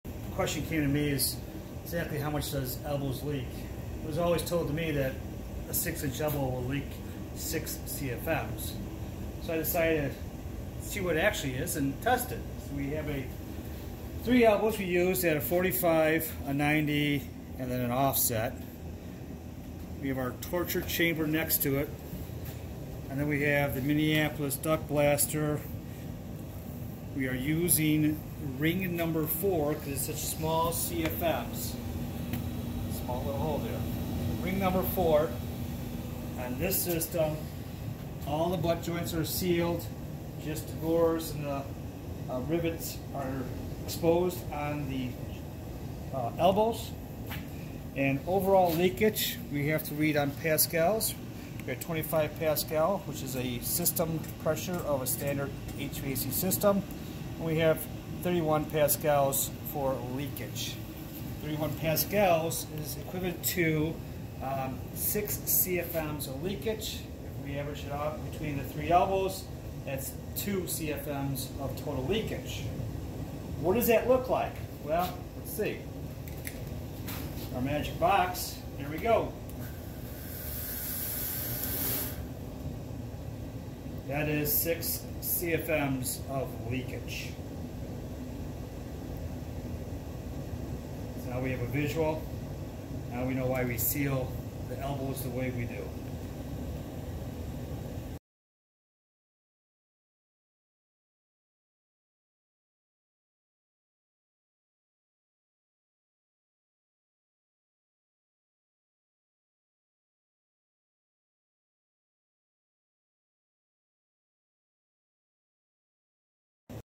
6 Inch Elbow Leakage Test
Testing how many CFM's do unsealed 6inch elbow actually leak?